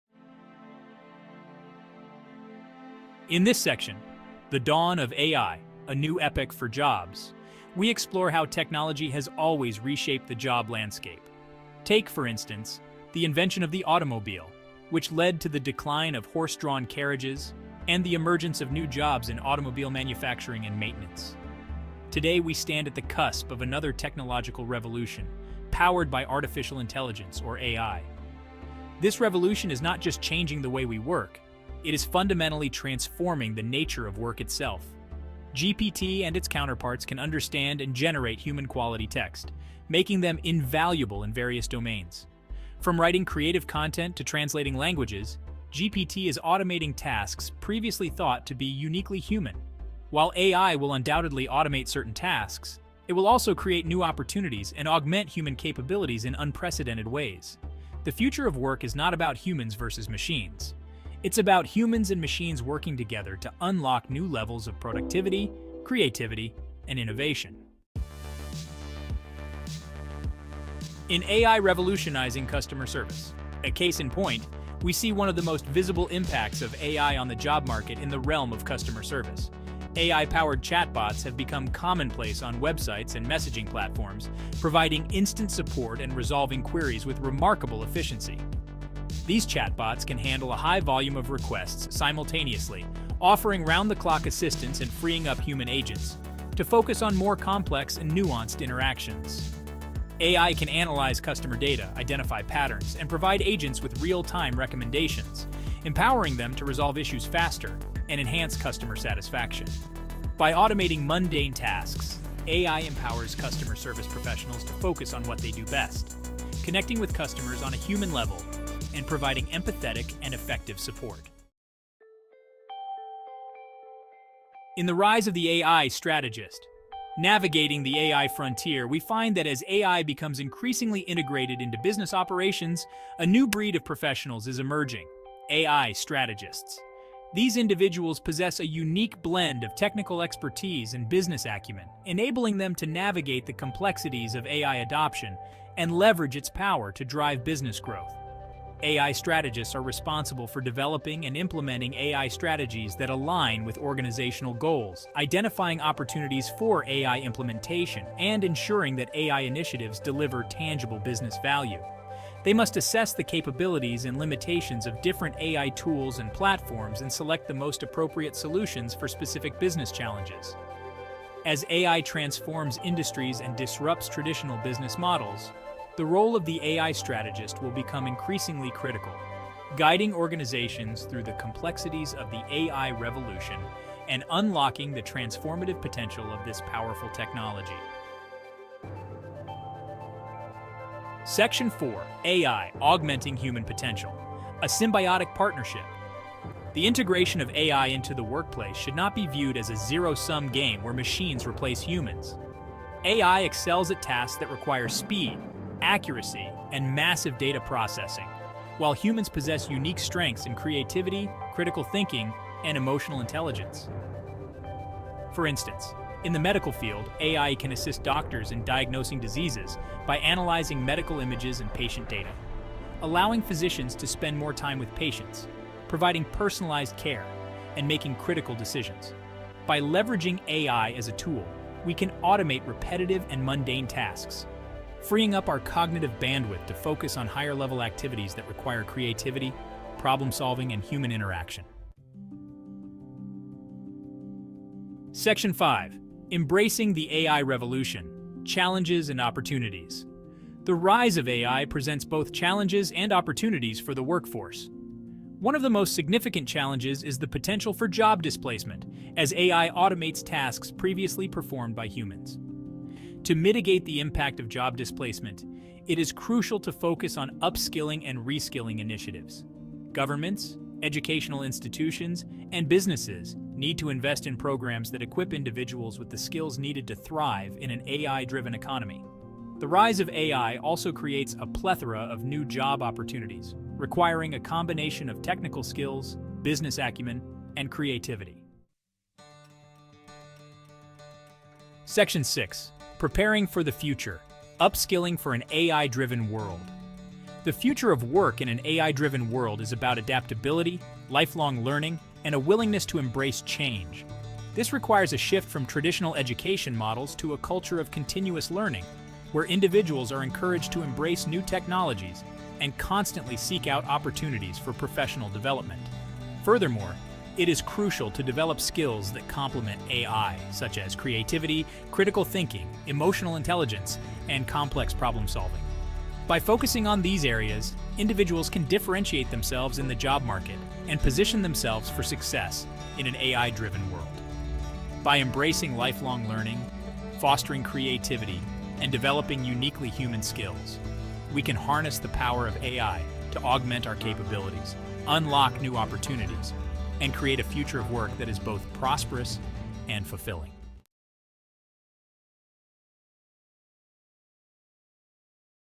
AI Unleashed: Reshaping Careers with GPT
Harnessing Power for the Future: GPT's Radical Workforce Transformation

Job markets worldwide are in an escalated state of evolution mainly driven by technological advancements, and particularly by innovations like Generative Pretrained Transformers (GPT). In a time that has seen AI systems train robots to outperform humans, which career paths lie ahead, and how do we navigate this excitingly futuristic job market?

GPT and similar innovations are creating a ripple effect in the workspace, blessing the world with real-life applications of AI technologies. From training robots to mimic a dog balancing on a yoga ball to strategic AI systems aiding customer service, AI's potential is vast.

With the automation of monotonous, human-handled tasks, the job market has seen a surge in demand for creative, strategic, and empathetic roles. AI strategists and emerging technology experts are now inevitable roles in organizations planning for an AI-centric future. Professionals using their knowledge to achieve the real-world applications these technologies promise will be pivotal in sectors like healthcare and finance.

As continuous innovation in technology becomes the norm, will jobs as we know them still exist? Or will we witness a merge between traditional jobs with a technological touch? Will customer service now mean humans aided by AI-powered bots?

The future jobs market seems promising despite the seemingly threatening view of technology taking over. A report suggests that AI-related jobs have seen a growth of 74% in the past four years, indicating the positive potential of AI technologies. With immense resources being invested in AI and automation, the transformation of the workforce is inevitable.

In the spirit of technological advancement providing immense potential to people's jobs and careers, the question remains - are we ready to embrace the future and the career opportunities it promises? and see how Klap uses AI to turn any video into viral shorts, promoting opportunities in the digital landscape.

 AI Unleashed: Reshaping Careers with GPT by Steven's Workspace

OUTLINE:

00:00:00 A New Epoch for Jobs
00:01:07 A Case in Point
00:02:01 Navigating the AI Frontier
00:03:08 A Symbiotic Partnership
00:04:01 Challenges and Opportunities
00:04:48 Upskilling for an AI-Driven World